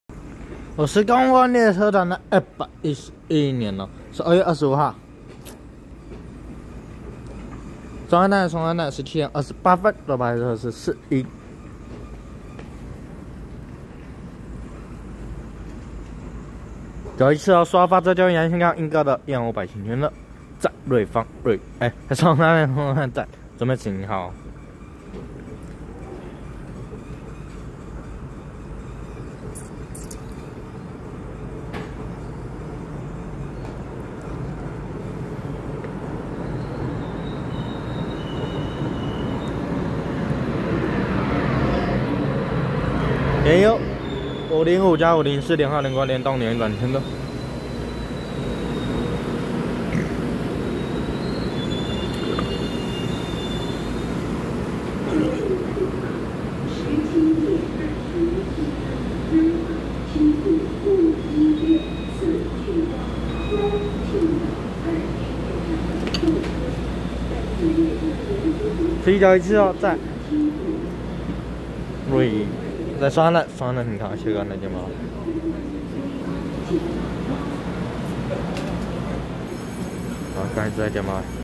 4191次蘇澳發車經由宜蘭線開往鶯歌的EMU505+EMU504連號連掛聯動聯運轉區間車
111/12/25松山站17:28分拍攝4191次蘇澳發車經由宜蘭線開往鶯歌的EMU505+EMU504連號連掛聯動聯運轉區間車